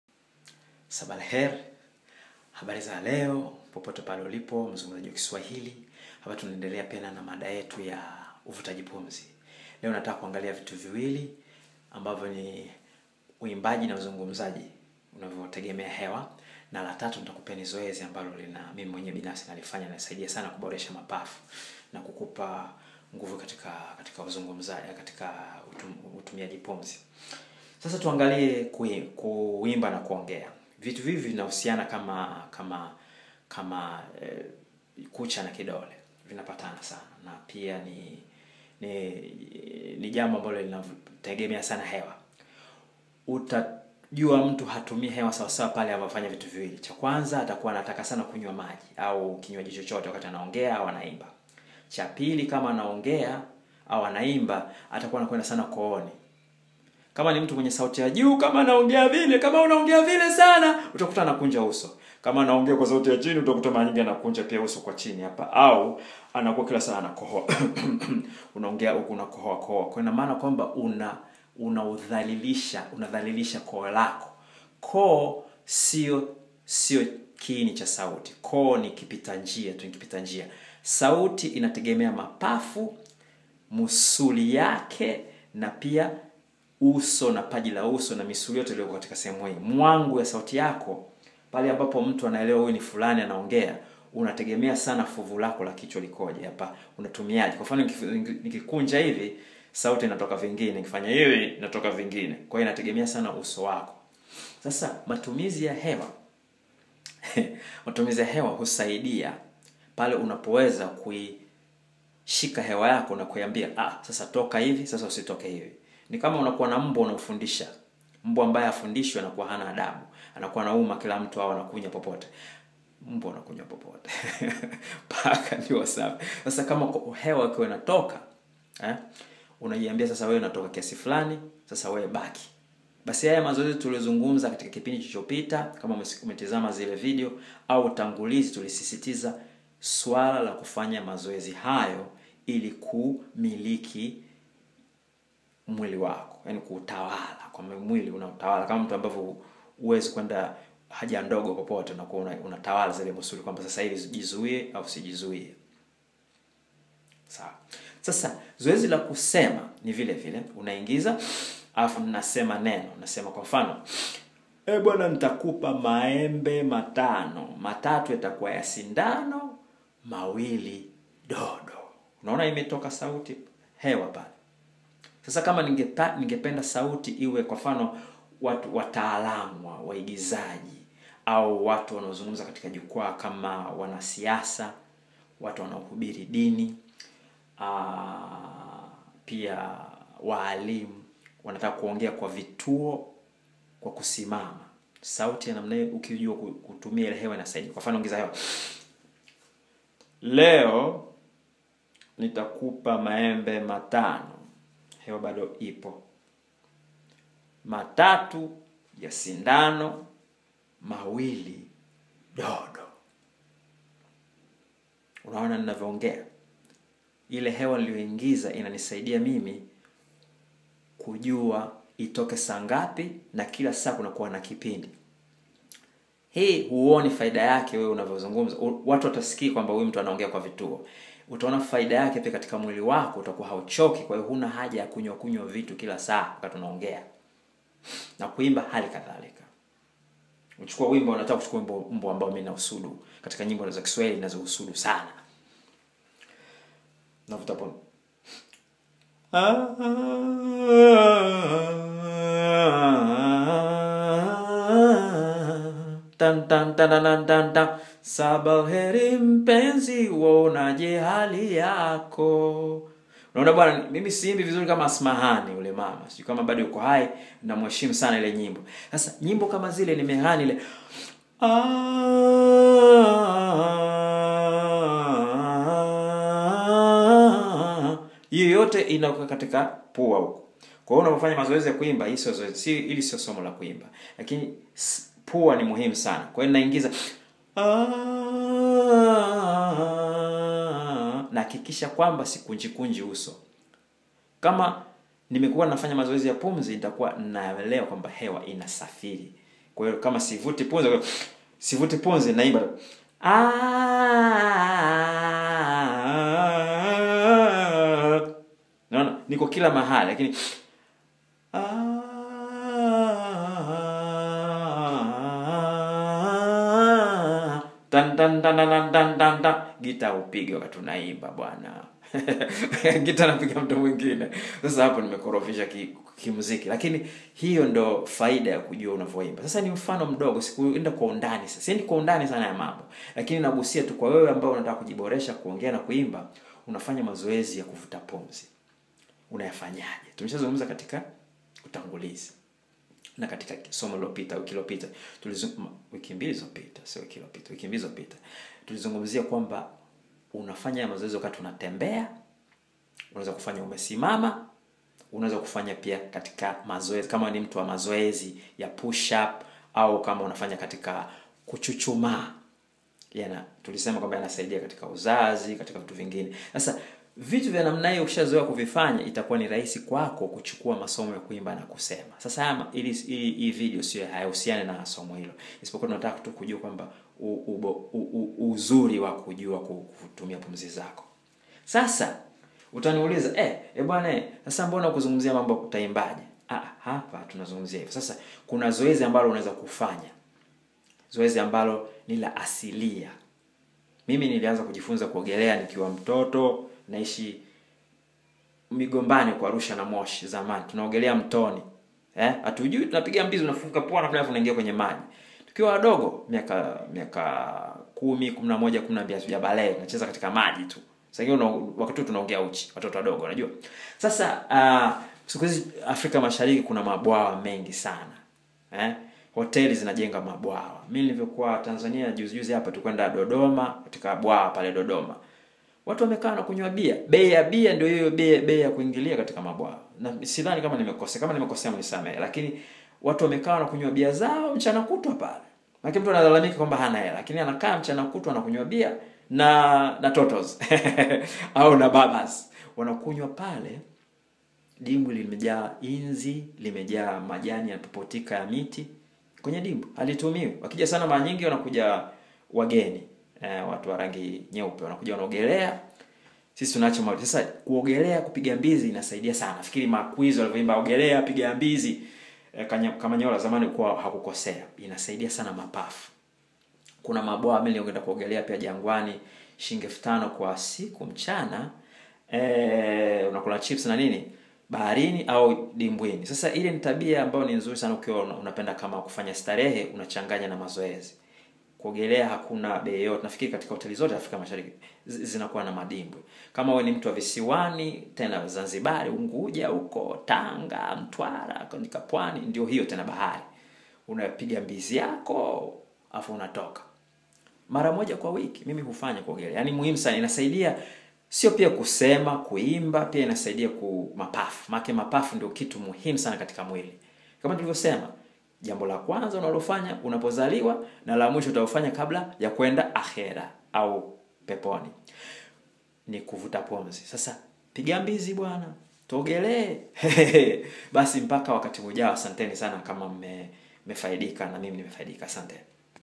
FAIDA ZA MAZOEZI YA UVUTAJI PUMZI -Zoezi la 2
Mwandishi na mwanamuziki Freddy Macha adokeza mambo mawili muhimu yanayofaidika na tizi la pumzi yaani kuimba na kuzungumza...